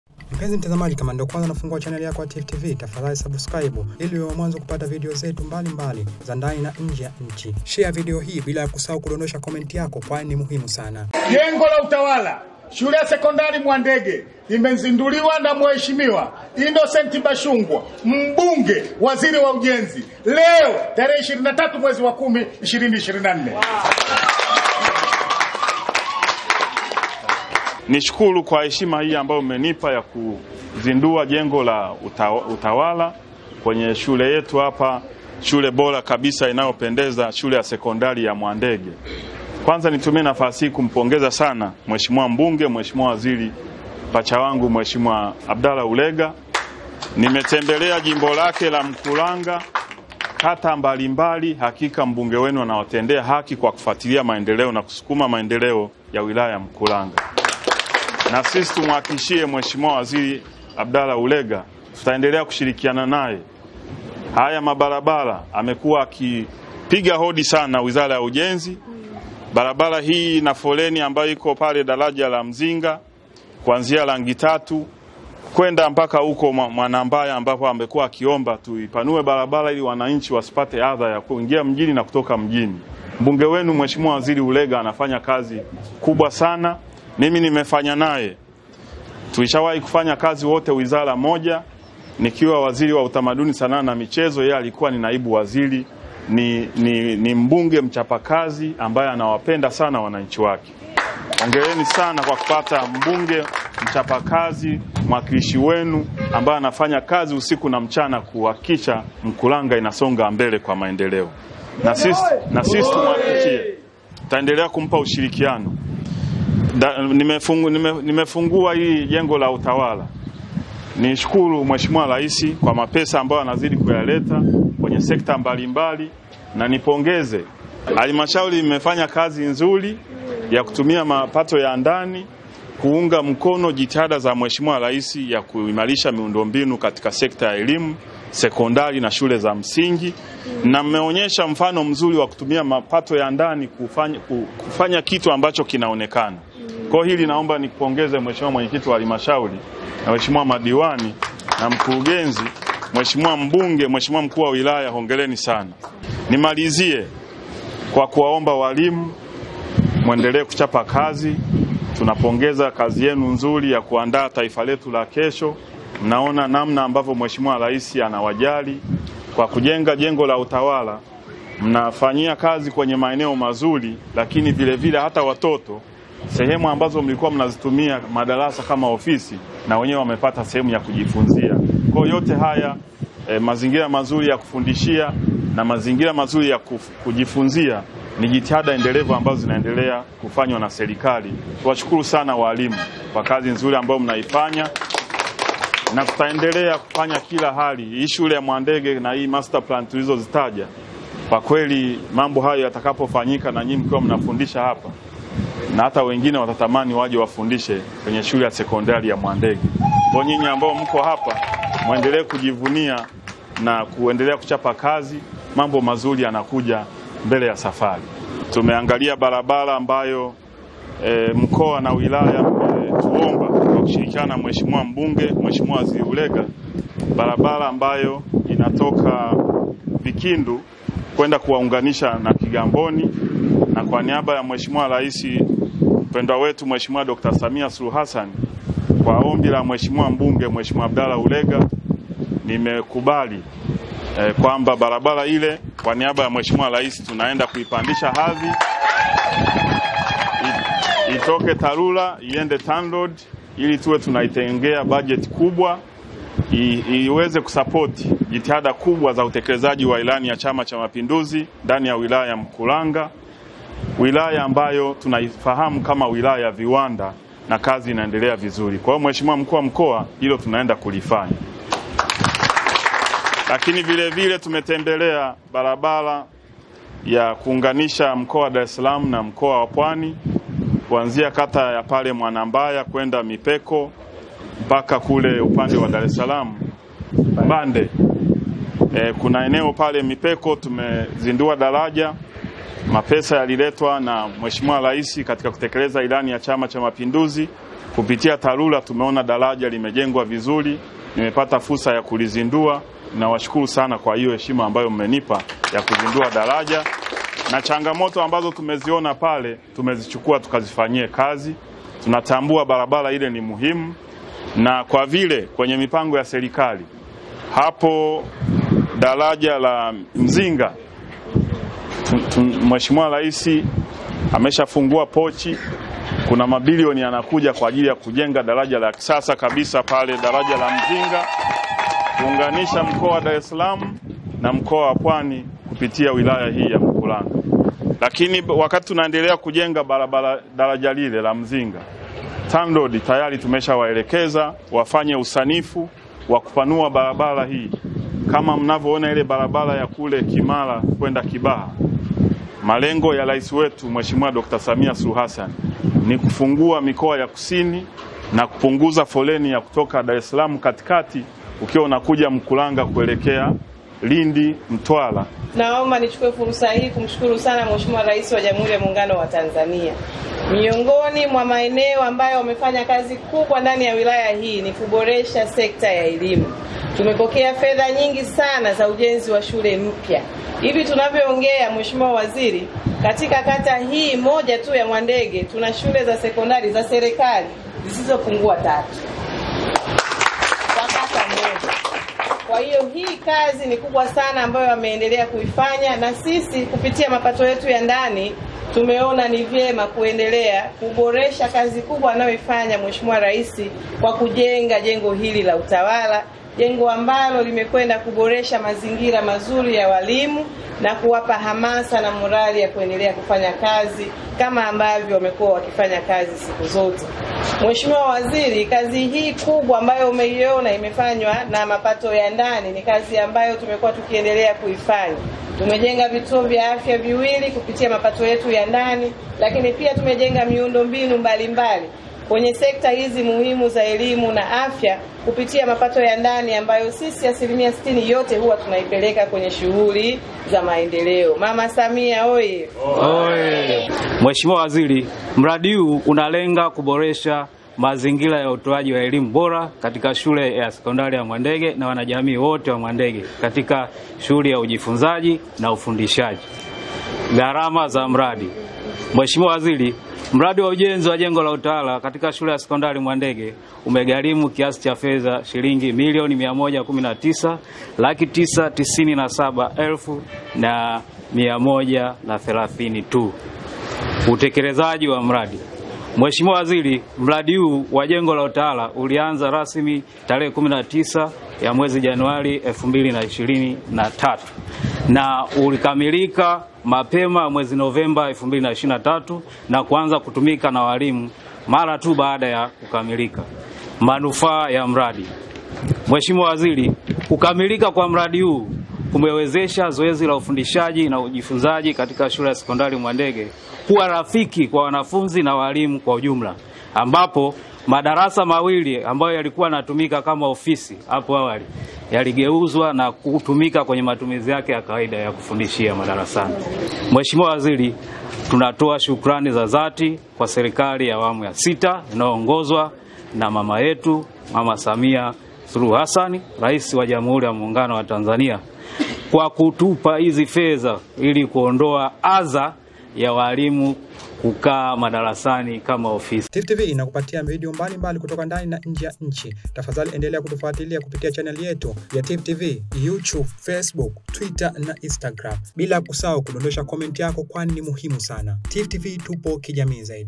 HII NDO MKURANGA-MILIONI 119.9 ZATUMIKA KUINGARISHA 2024
Waziri wa Ujenzi, Innocent Bashungwa amezindua jengo la utawala katika Shule ya Sekondari ya Mwandege lililojengwa na kukamilika kupitia fedha za mapato ya ndani ya Halmashauri ya Wilaya ya Mkuranga lililogharimu kiasi cha Shilingi Milioni 119.9.

Bashungwa amezindua jengo hilo leo Oktoba 23,   2024 Wilayani Mkuranga Mkoani Pwani ikiwa ni muendelezo wa ziara yake maalum katika Mkoa huo ya kukagua utekelezaji na kuzindua miradi ya maendeleo pamoja na kuzungumza na wananchi.

Bashungwa amemshukuru Mhe. Rais Dkt. Samia Suluhu Hassan kwa kuendelea kutoa fedha za miradi ya maendeleo kwenye Sekta mbalimbali katika Mkoa wa Pwani na ameipongeza Halmashauri ya Wilaya ya Mkuranga kwa kazi nzuri ya usimamizi wa fedha hizo.

Katika hatua nyingine, Waziri Bashungwa amezindua daraja katika barabara ya Mwanambaya – Mipeko eneo la bonde la mto Mzinga lililojengwa na kusimamiwa  na Wakala ya Barabara za Mijini na Vijijini (TARURA) kwa ajili ya kuimarisha miundombinu ya barabara na kuwezesha mawasiliano katika Kata ya Mipeko, Mwanambaya na Mbande ili kurahisisha shughuli za kiuchumi na huduma za kijamii kwa Wilaya za Mkuranga na Temeke.

Aidha, Bashungwa ameeleza kuwa Serikali inaendelea kumtafuta Mkandarasi kwa ajili ya kujenga daraja la kisasa la Mto Mzinga ili kusaidia kupunguza foleni katika barabara kuu ya Dar es Salaam - Lindi hususan eneo la Mbagala.

Bashungwa amepokea ombi la Mkuu wa Mkoa wa Pwani la kupandisha hadhi barabara ya Vikindu - Vianzi - Sangatini inayounganisha Wilaya ya Mkuranga na Kigamboni ili iweze kuhudumiwa na Wakala ya Barabara (TANROADS).

Vilevile, Bashungwa amemuelekeza Kaimu Mtendaji Mkuu wa TANROADS, Eng. Dorothy Mtenga kuhakikisha wanashirikiana na Ofisi ya  Mkuu wa Pwani katika usanifu wa upanuzi wa barabara za Mkoa huo ili ziende sambamba na mikakati na mipango ya uboreshaji wa Mkoa ikiwemo ujenzi wa Stendi ya Kisasa ya Mabasi inayotarajiwa kujengwa Mkuranga.

Kwa upande wake, Mkuu wa Mkoa wa Pwani, Abubakar Kunenge amemshukuru Mhe. Rais Dkt. Samia kwa kuendelea kutoa fedha  za miradi mbalimbali ambapo kiasi cha Shilingi Bilioni 121.1 zimepokelewa kwa ajili ya utekelezaji wa miundombinu ya barabara zinazosimamiwa na TANROADS na TARURA.

Naye, Mkuu wa Wilaya ya Mkuranga, Khadija Ali ameeleza kuwa Wilaya hiyo imepokea fedha nyingi za ujenzi wa shule mpya ambapo katika Kata ya Mwandege Shule za Sekondari za Serikali tatu.

Akiwasilisha taarifa ya mradi wa Jengo la Utawala, Mkuu wa Shule ya Sekondari ya Mwandege Bw. Ditrick Hokororo ameeleza kuwa kukamilika kwa jengo hilo kumewezesha zoezi la ufundishaji na ujifunzaji katika Shule hiyo kuwa rafiki kwa wanafunzi na walimu ambapo madarasa mawili yaliyokuwa yanatumika kama ofisi za walimu yamerejeshwa kwenye matumizi ya kawaida kwa wanafunzi.